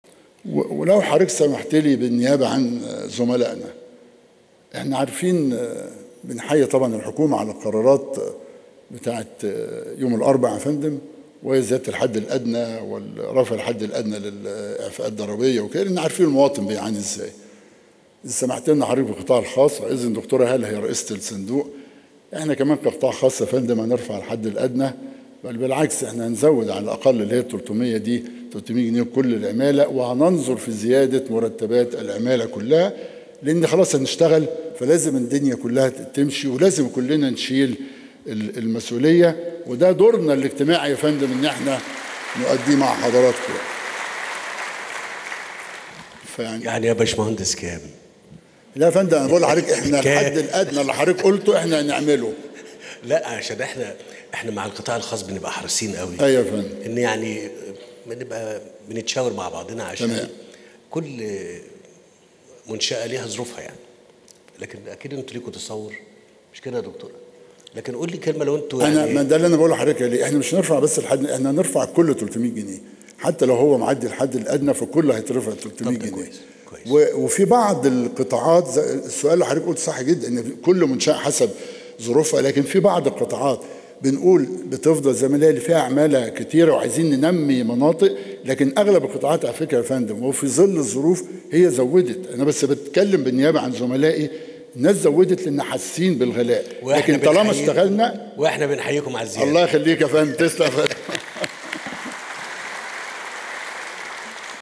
رئيس اتحاد الصناعات المصرية: زيادة مرتبات القطاع الخاص 300 جنيه
أخبار أون
تغطية فعاليات إطلاق الملتقى الدولى الأول للصناعة بحضور الرئيس السيسي